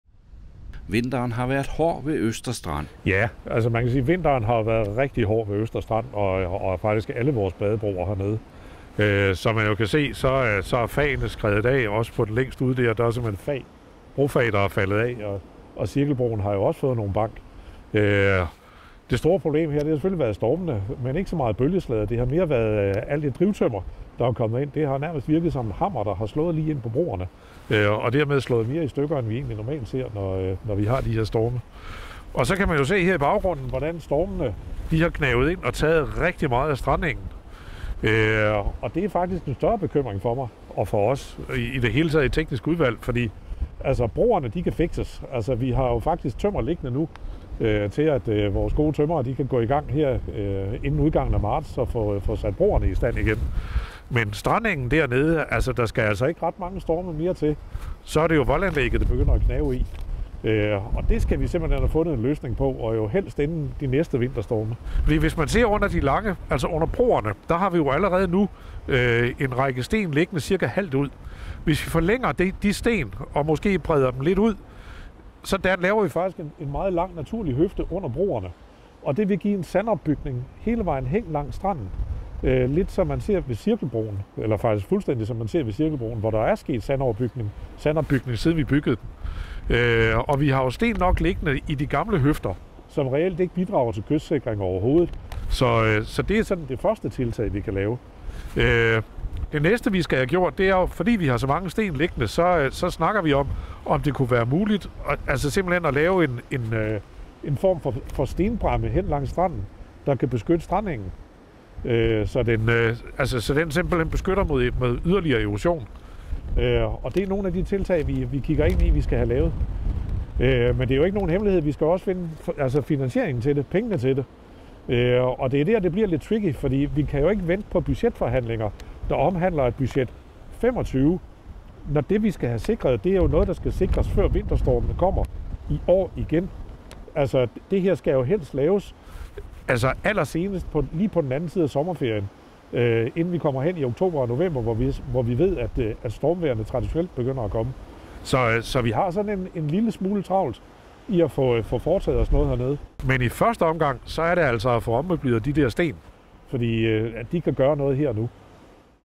Vinteren har været hård ved Østerstrand, og volden er ved at være truet
Bølgerne har ikke været det værste, men derimod rigtig meget drivtømmer, der har slået ind mod broerne, fortæller Christian Bro fra Teknisk Udvalg.